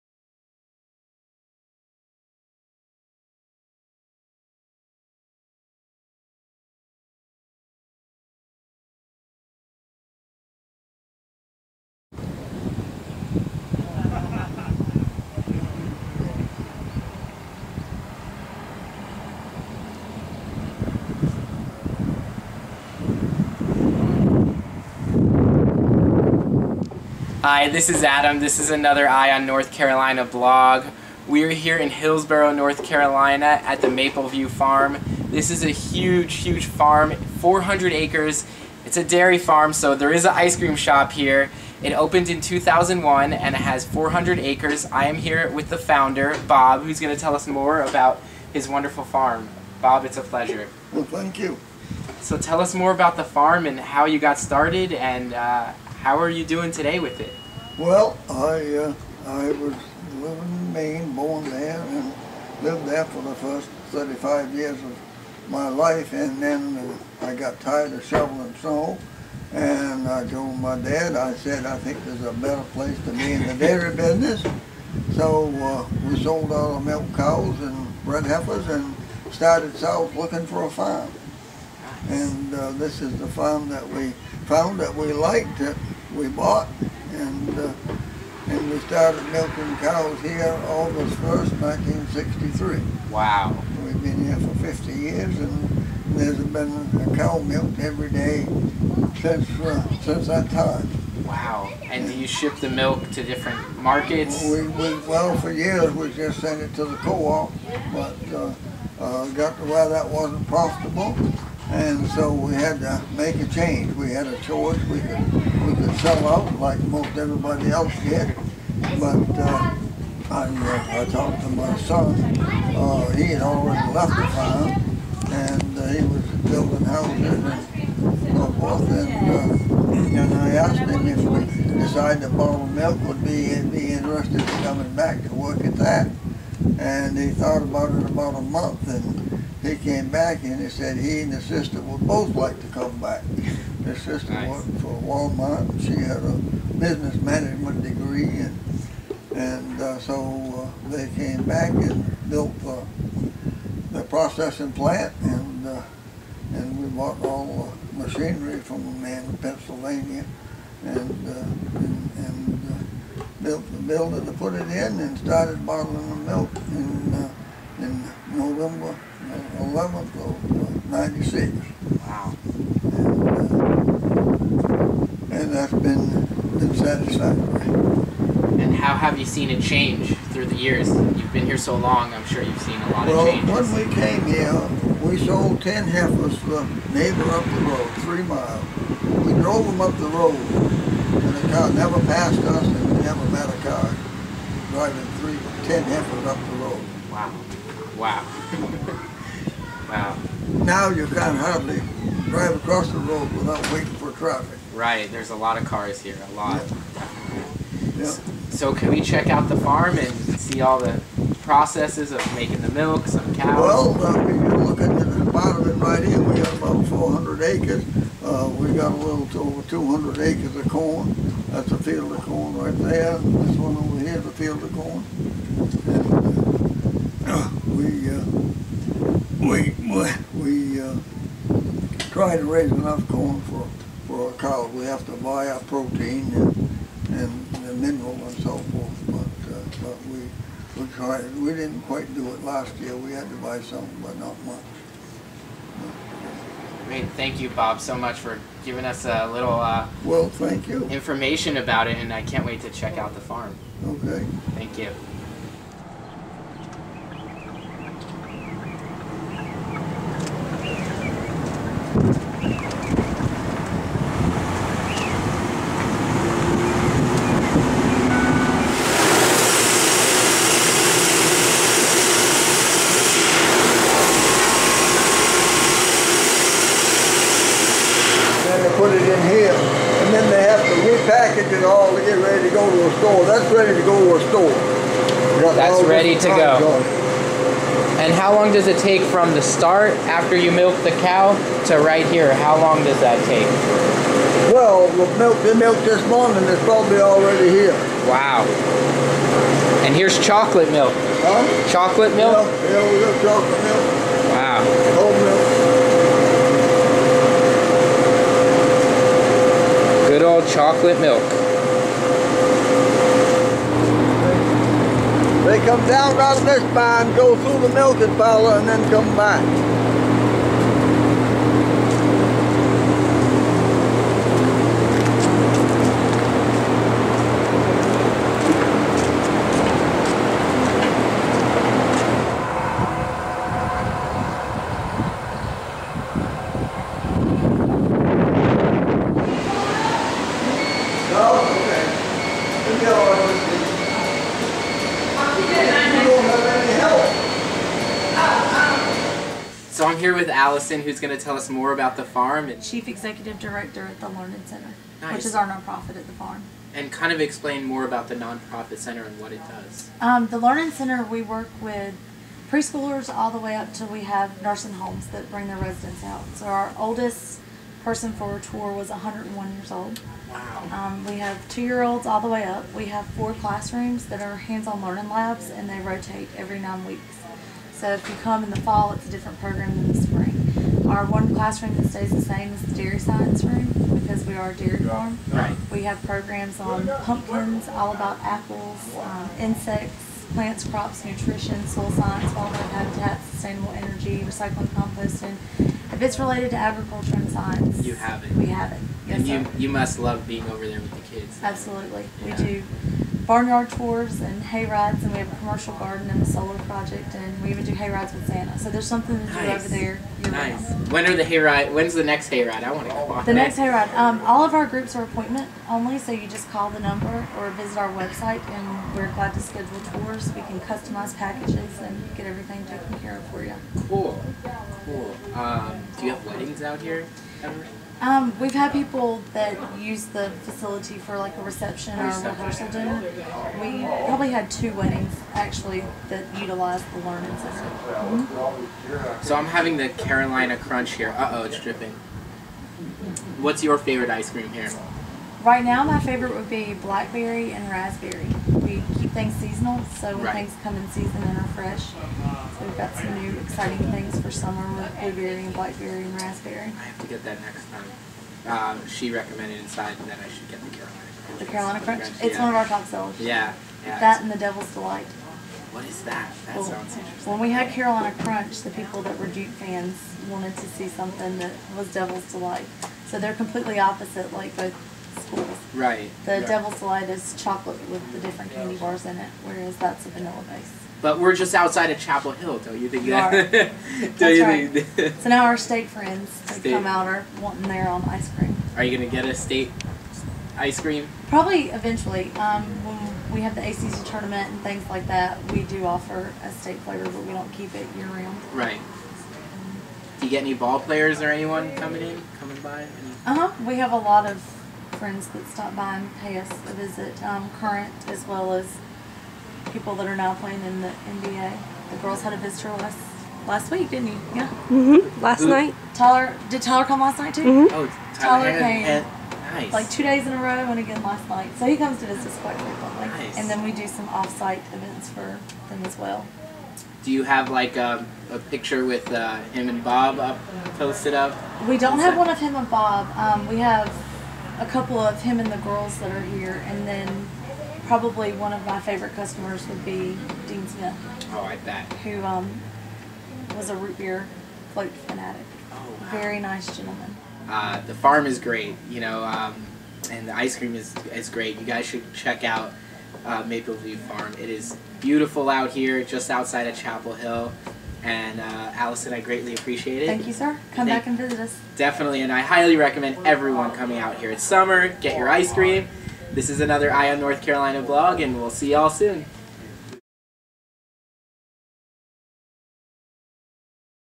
Maple View Farm - HIllsborough, North Carolina
I traveled out to Maple View Farm for delicious ice cream and a tour of the farm with Farmer Bob and Allison.

Thank you for the tour!